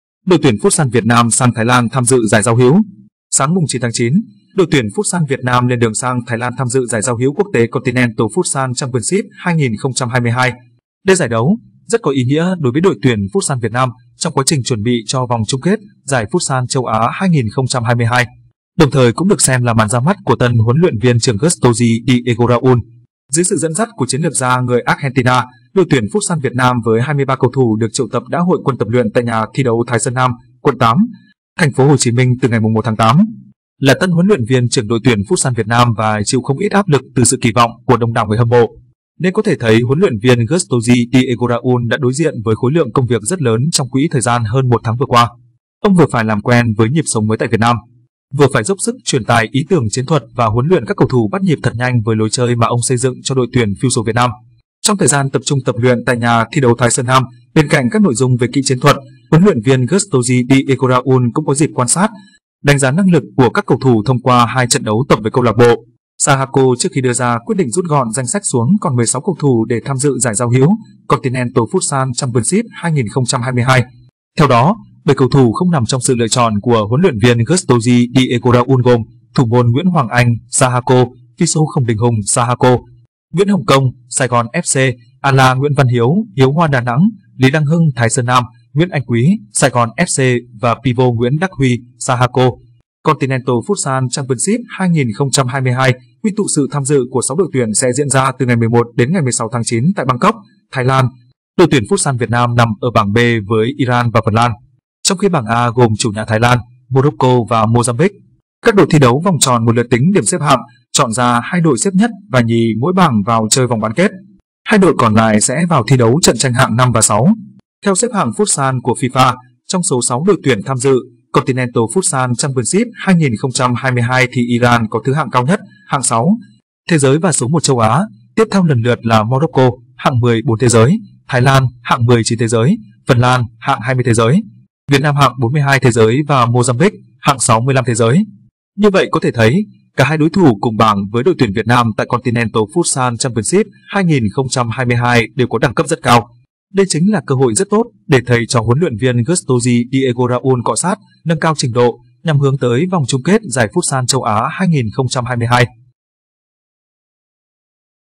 Đội tuyển futsal Việt Nam sang Thái Lan tham dự giải giao hữu
Sáng 9/9, đội tuyển futsal Việt Nam lên đường sang Thái Lan tham dự giải giao hữu quốc tế Continental Futsal Championship 2022. Đây giải đấu rất có ý nghĩa đối với đội tuyển futsal Việt Nam trong quá trình chuẩn bị cho Vòng chung kết Giải futsal châu Á 2022, đồng thời cũng được xem là màn ra mắt của tân huấn luyện viên trưởng Gustozzi Diego Raul.Ông vừa phải làm quen với nhịp sống mới tại Việt Nam, vừa phải dốc sức truyền tải ý tưởng chiến thuật và huấn luyện các cầu thủ bắt nhịp thật nhanh với lối chơi mà ông xây dựng cho đội tuyển fusal Việt Nam.
Là tân huấn luyện viên trưởng đội tuyển futsal Việt Nam và chịu không ít áp lực từ sự kỳ vọng của đông đảo người hâm mộ, nên có thể thấy huấn luyện viên Gustozzi Diego Raul đã đối diện với khối lượng công việc rất lớn trong quỹ thời gian hơn 1 tháng vừa qua.
Continental Futsal Championship 2022 quy tụ sự tham dự của 6 đội tuyển sẽ diễn ra từ ngày 11 đến 16/9 tại Bangkok, Thái Lan. Đội tuyển futsal Việt Nam nằm ở bảng B với Iran và Phần Lan. Trong khi bảng A gồm chủ nhà Thái Lan, Morocco và Mozambique.
Đội tuyển futsal Việt Nam chụp ảnh lưu niệm tại sân bay Tân Sơn Nhất (TP Hồ Chí Minh) trước giờ khởi hành sang Thái Lan. (Ảnh: VFF)
Theo đó, 7 cầu thủ không nằm trong sự lựa chọn của huấn luyện viên Gustozzi Diego Raul gồm: thủ môn Nguyễn Hoàng Anh (Sahako), fixo Khổng Đình Hùng (Sahako), Nguyễn Hồng Kông (Sài Gòn FC), ala Nguyễn Văn Hiếu (Hiếu Hoa Đà Nẵng), Lý Đăng Hưng (Thái Sơn Nam), Nguyễn Anh Quý (Sài Gòn FC) và pivo Nguyễn Đắc Huy (Sahako).
Như vậy có thể thấy, cả 2 đối thủ cùng bảng với đội tuyển Việt Nam tại Continental Futsal Championship 2022 đều có đẳng cấp rất cao. Đây chính là cơ hội rất tốt để thầy trò huấn luyện viên Gustozzi Diego Raul cọ xát, nâng cao trình độ, nhằm hướng tới Vòng chung kết giải futsal châu Á 2022.
Trong thời gian tập trung tập luyện tại Nhà thi đấu Thái Sơn Nam, bên cạnh các nội dung về kỹ chiến thuật, huấn luyện viên Gustozzi Diego Raul cũng có dịp quan sát, đánh giá năng lực của các cầu thủ thông qua 2 trận đấu tập với câu lạc bộ Sahako trước khi đưa ra quyết định rút gọn danh sách xuống còn 16 cầu thủ để tham dự giải giao hữu Continental Futsal Championship 2022.
Các đội thi đấu vòng tròn 1 lượt tính điểm xếp hạng, chọn ra 2 đội xếp nhất và nhì mỗi bảng vào chơi vòng bán kết. Hai đội còn lại sẽ vào thi đấu trận tranh hạng 5 và 6.
Dưới sự dẫn dắt của chiến lược gia người Argentina, đội tuyển futsal Việt Nam với 23 cầu thủ được triệu tập đã hội quân tập luyện tại Nhà thi đấu Thái Sơn Nam, quận 8, TP Hồ Chí Minh từ ngày 1/8.
Theo xếp hạng futsal của FIFA, trong số 6 đội tuyển tham dự Continental Futsal Championship 2022 thì Iran có thứ hạng cao nhất (hạng 6 thế giới và số 1 châu Á), tiếp theo lần lượt là Morrocco (hạng 14 thế giới), Thái Lan (hạng 19 thế giới), Phần Lan (hạng 20 thế giới), Việt Nam hạng 42 thế giới và Mozambique (hạng 65 thế giới).